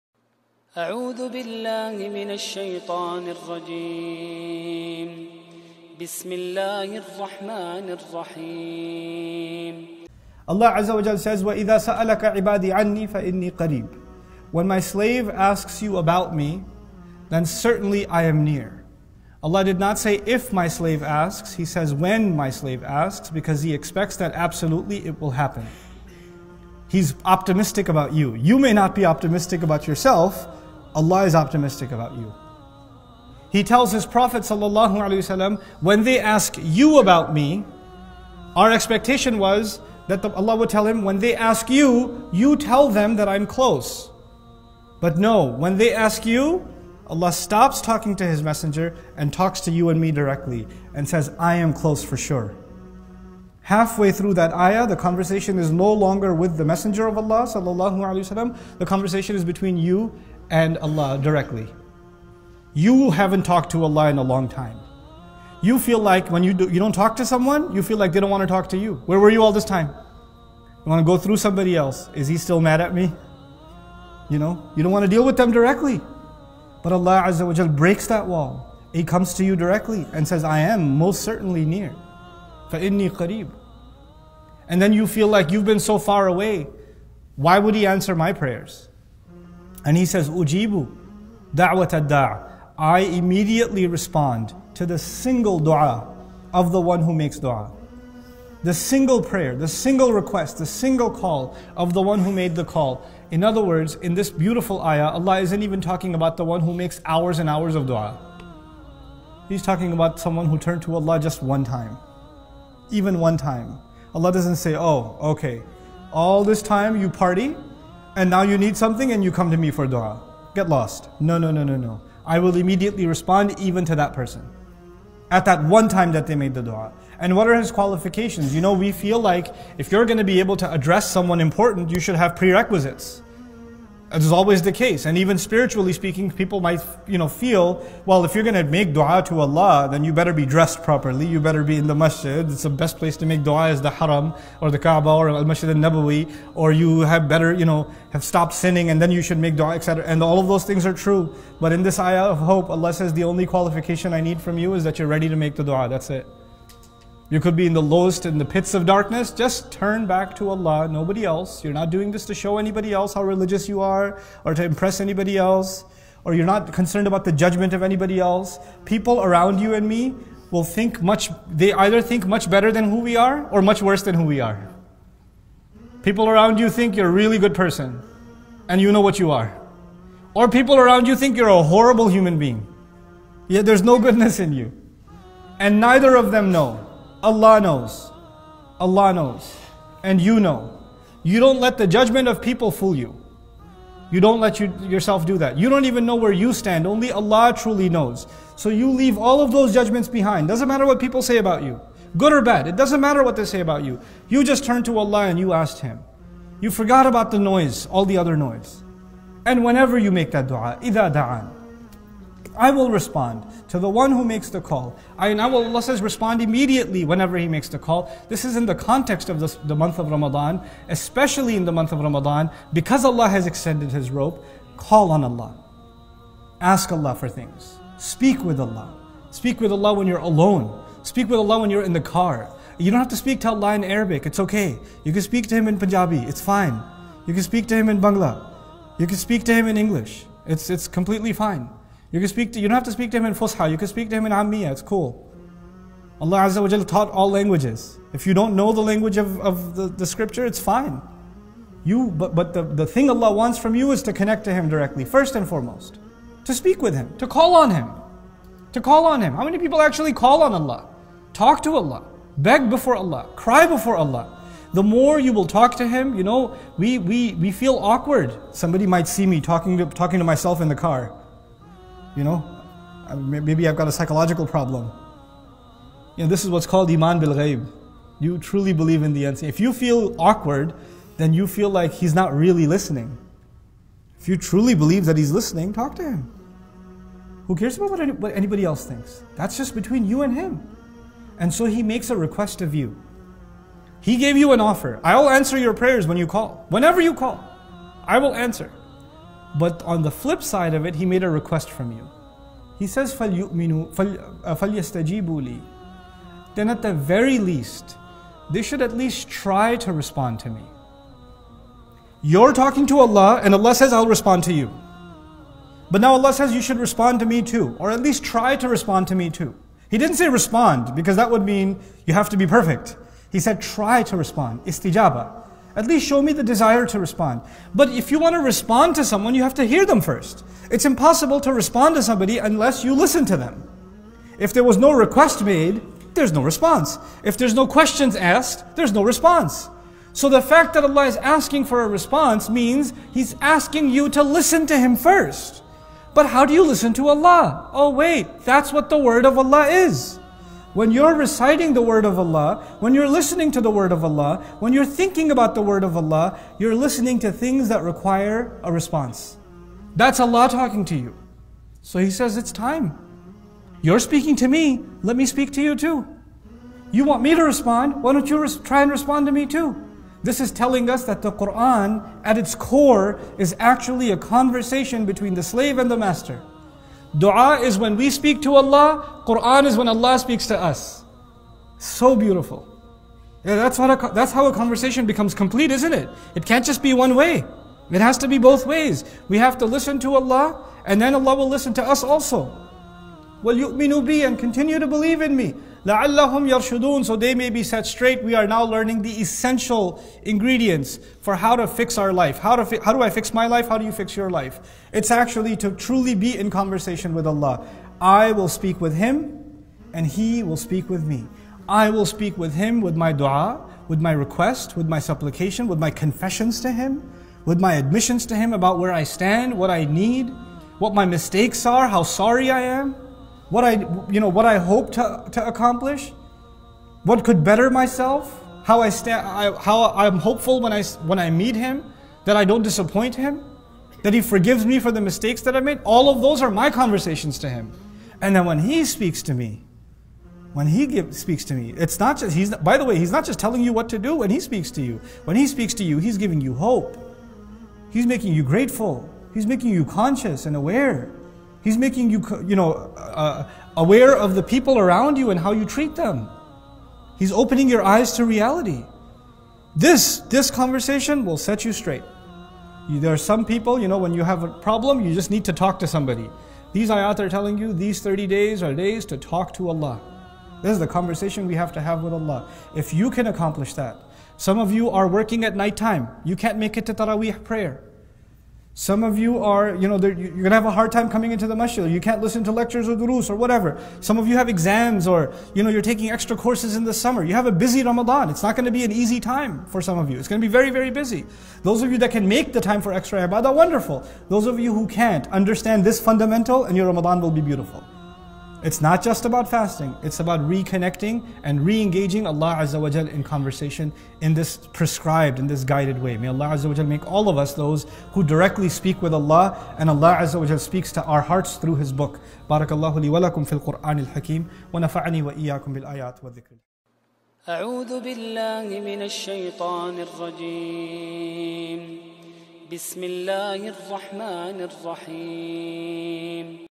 "TALK TO ALLAH" by Nouman Alikhan (Ramadhan Edition) (Islamic Speech)
"TALK TO ALLAH"
(RAMADHAN EDITION)
8 Minutes Islamic Speech by Nouman Alikhan
Thank you for supporting our platform, I hope this video will inspire us and make us better Muslims. See you in another video
Wassalaamu Alaikum Warahmtullahi Wabaraakatuh